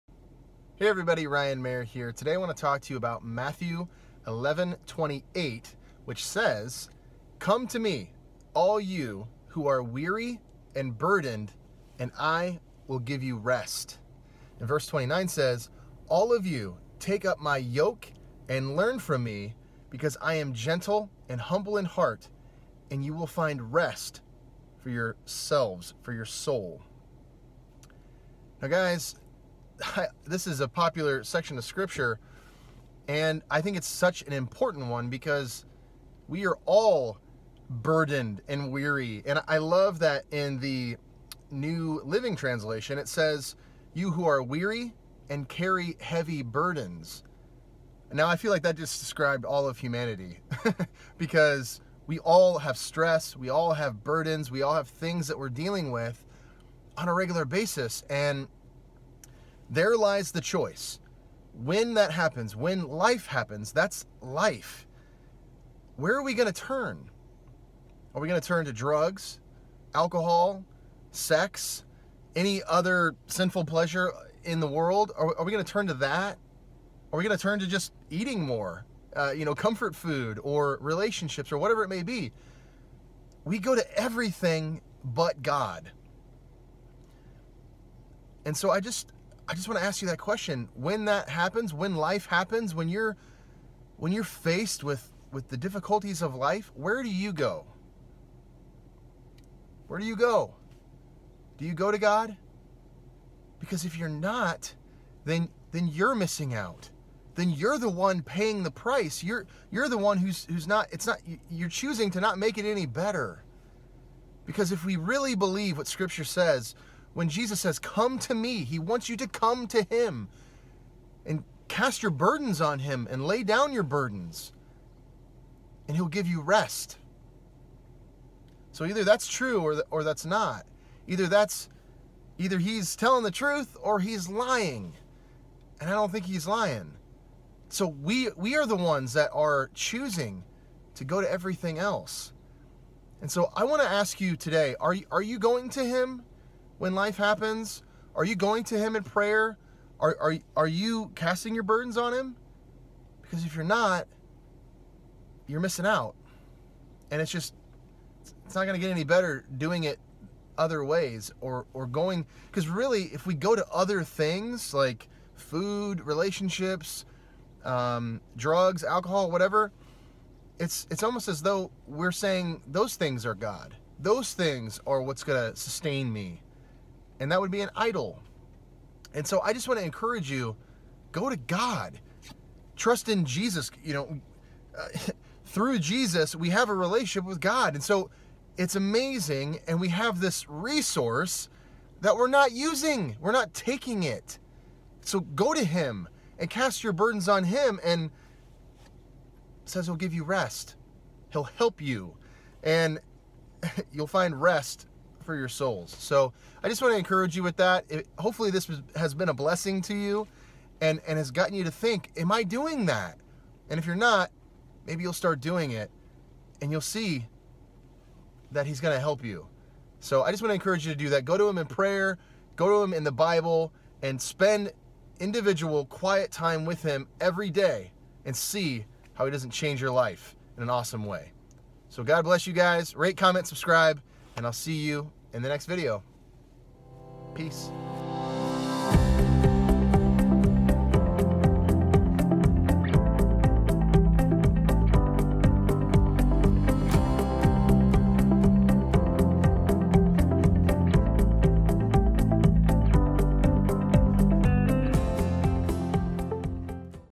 Cast your Burdens on the Lord - Matthew 11:28 - Ryan Maher
Where do you turn when the burdens of life happen? Stress, anxiety, family problems, money problems, etc. This are very common problems that are a part of life. So where do you turn when the troubles of life show up? Let's turn to the one source that can truly help: GOD.